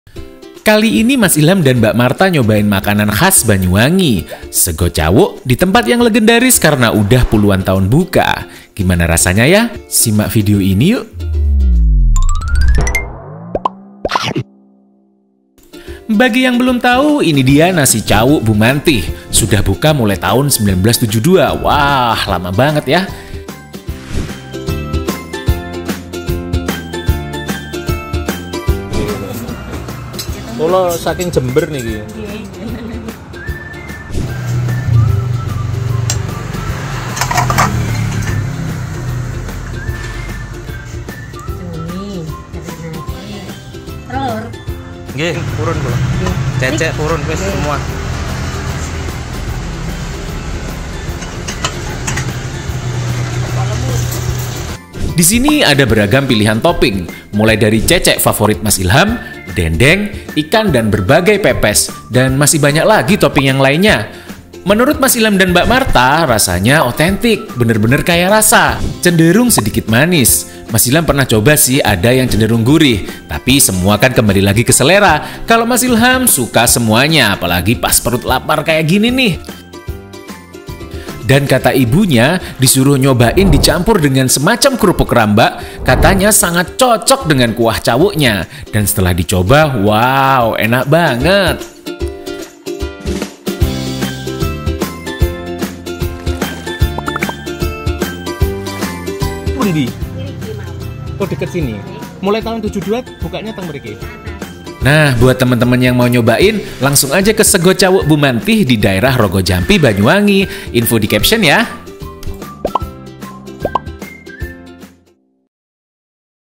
kuliner legendaris banyuwangi - sego cawuk bu mantih - 1972
Kuliner legendaris banyuwangi "sego cawuk bu mantih", sudah mulai tahun 1972, otentik rasanya 🔥

📍google maps - sego cawuk bu mantih - kalibaru - banyuwangi

Saran donk man teman, kuliner apalagi nih yang harus mas ilham dan mbak martha cobain 🤔kuliner